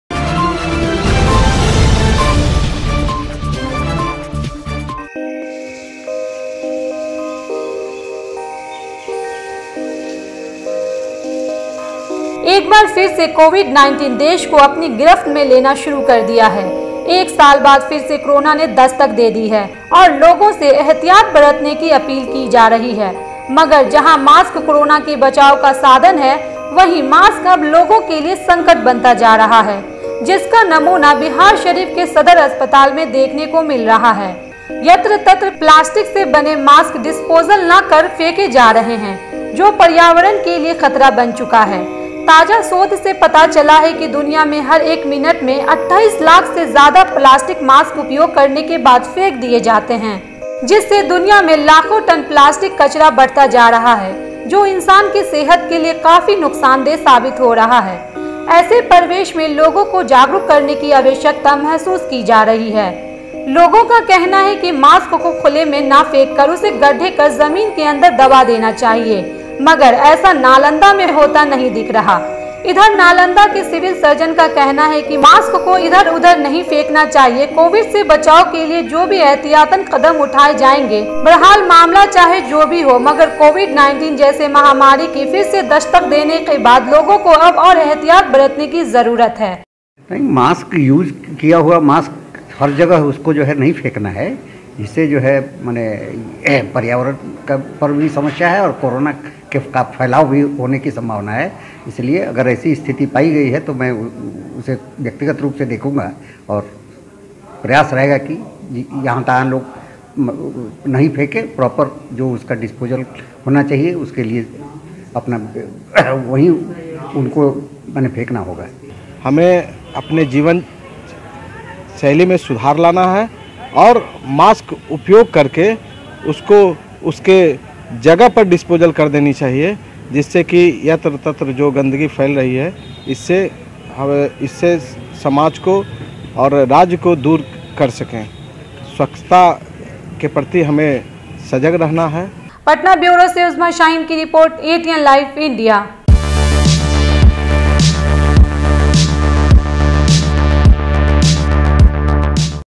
लोगों के लिए मास्क बनता जा रहा संकट ...ATN LIVE INDIA
लोगों को जागरूक करने की आवश्यकता...Nalanda...Biahr